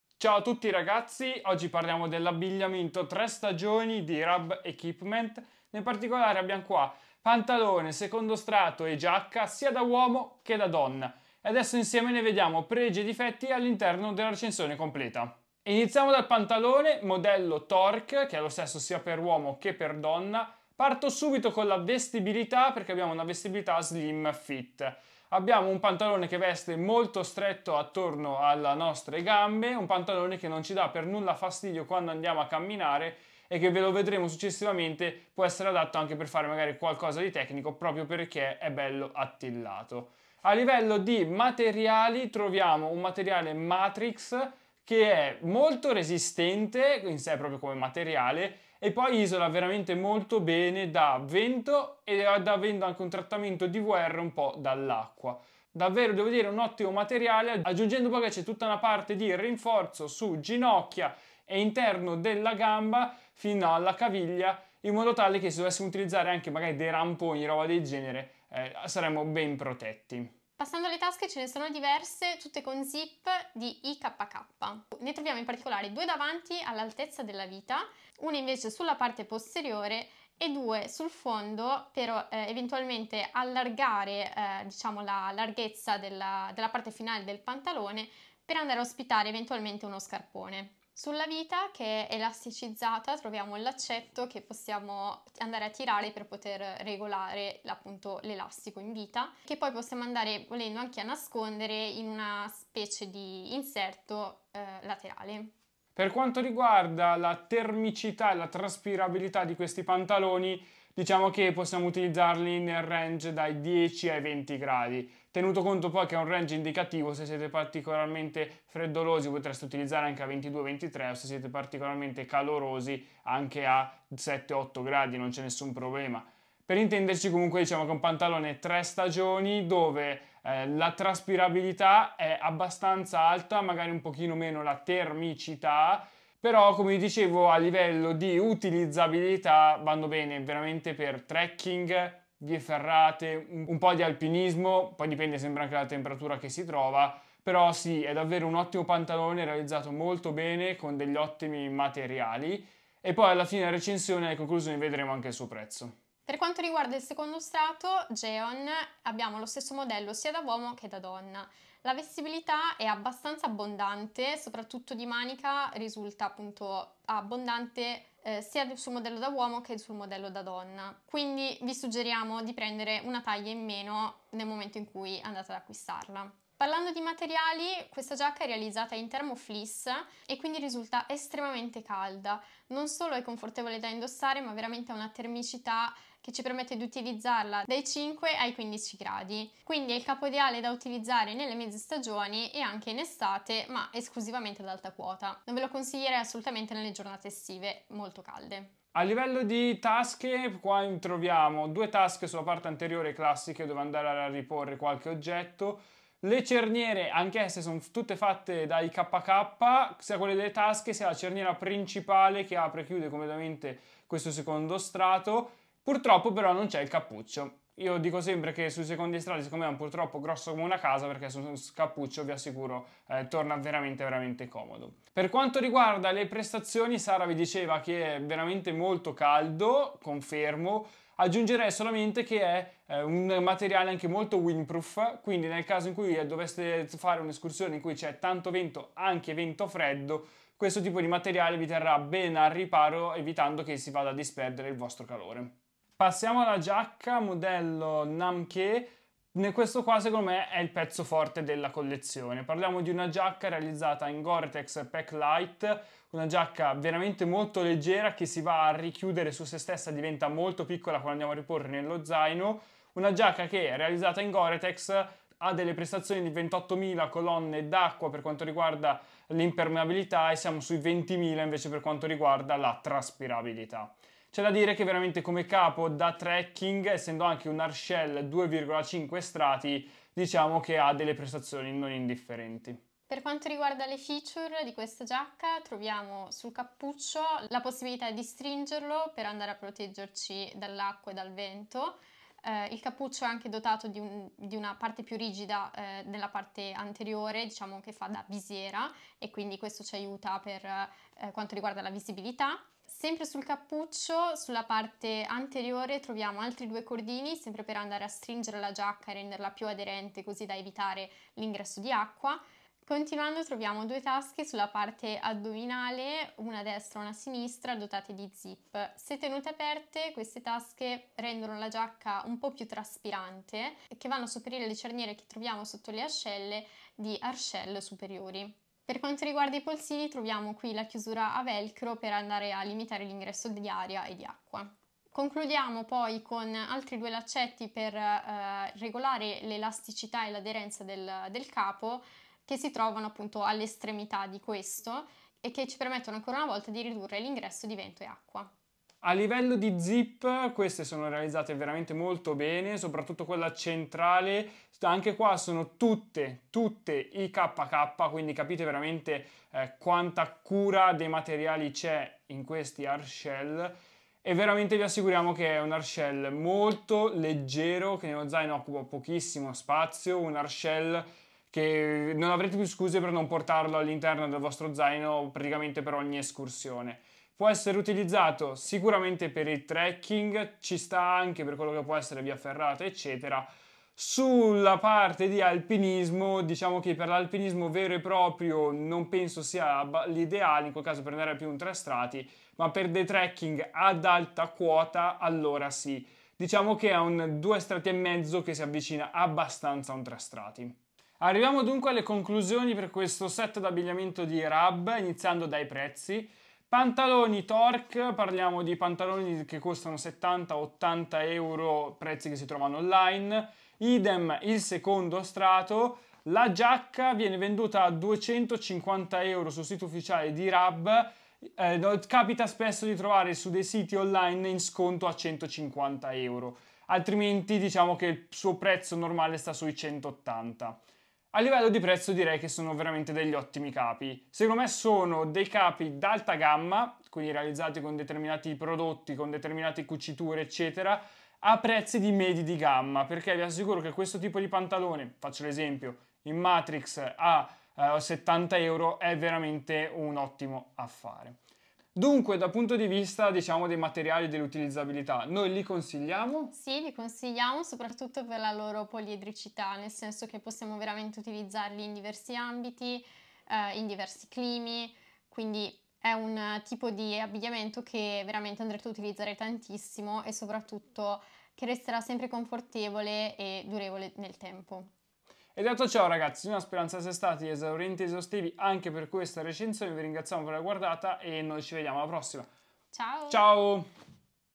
Recensione Abbigliamento 3 Stagioni Rab Equipment | Abbigliamento TOP a prezzi MEDI
Oggi parliamo dell'abbigliamento 3 stagioni di Rab Equipment.
Abbigliamento da trekking e via ferrata di cui vedremo pantaloni, secondo strato e hardshell.
Buona visione!